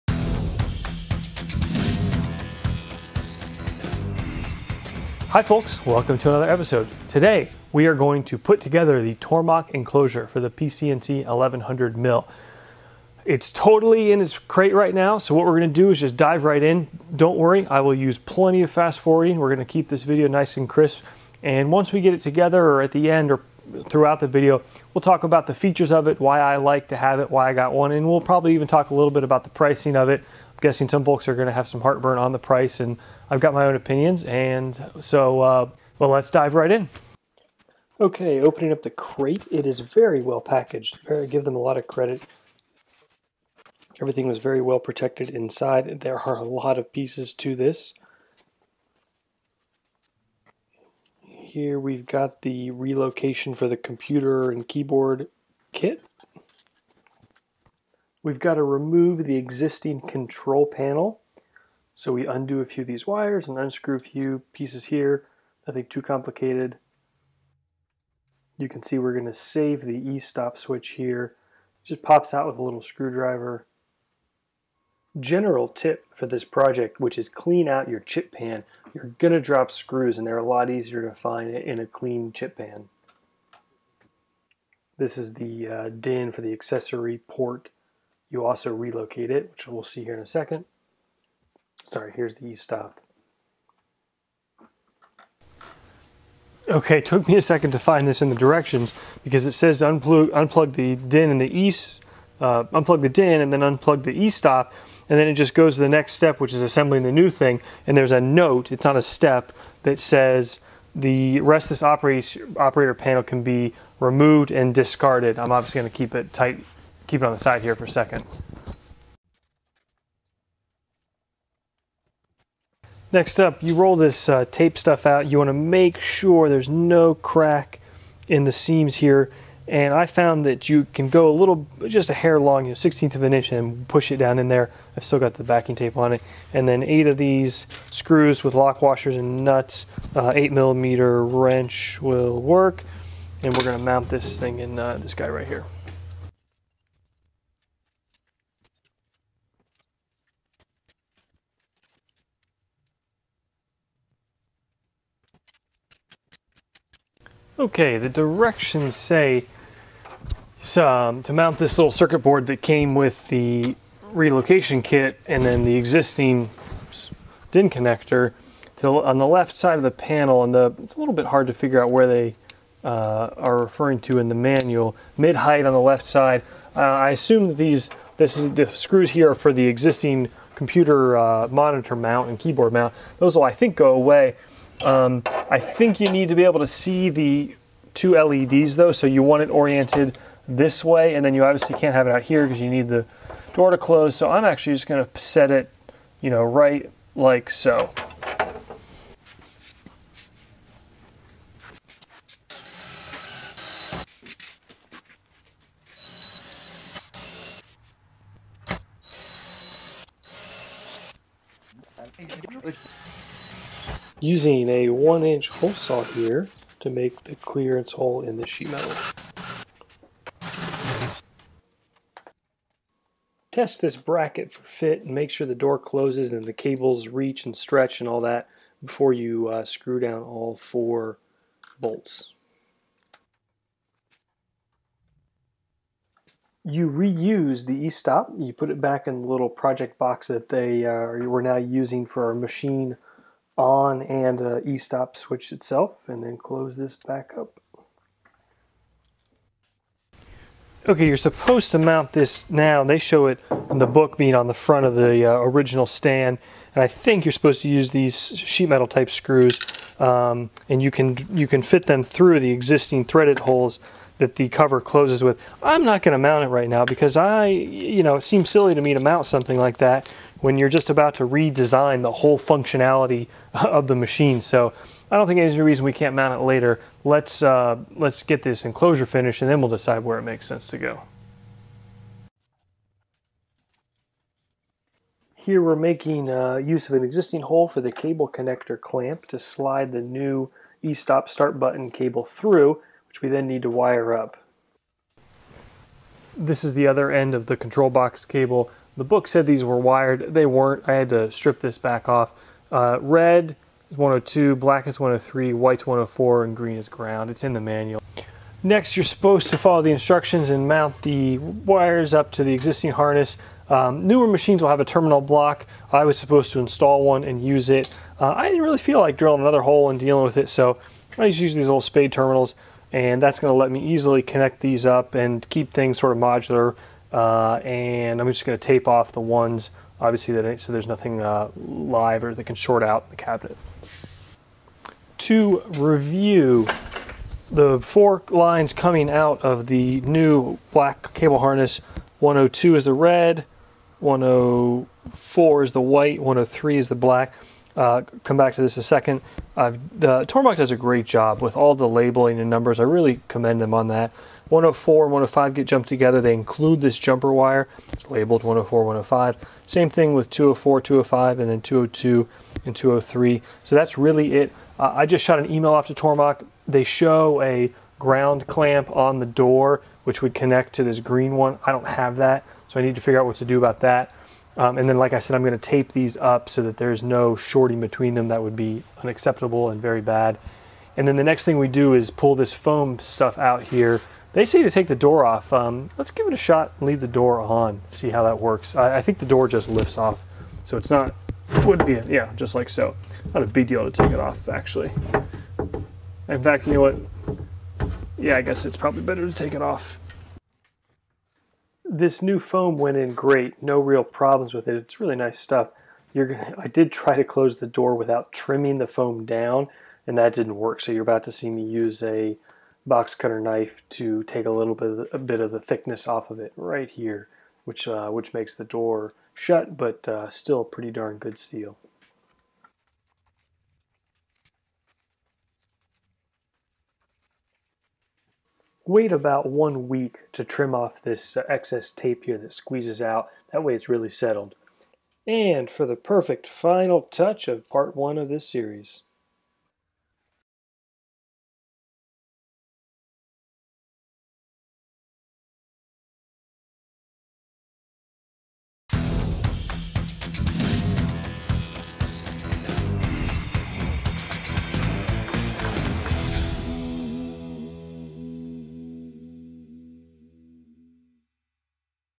Tormach PCNC Enclosure - Installation & Review Part 1
The chips must be contained!  This video covers the installation of the PCNC 1100 Full Enclosure.

It's a great addition to the PCNC mill.  Made from 16 gauge powder coated sheet metal with large front door that easily open to over 4 feet, 1/8" polycarbonate windows (they're beautiful), built-in lighting and the requisite control relocation kit.

Installation was quite easy.  I'd recommend having a friend help and budgeting one full work day.  I'd also HIGHLY recommend the following tools:

DeWalt 18V Drill & Impact Gun
Greenlee Drill/Tap Combo
90 degree Ratchet Screwdriver
Bubble Level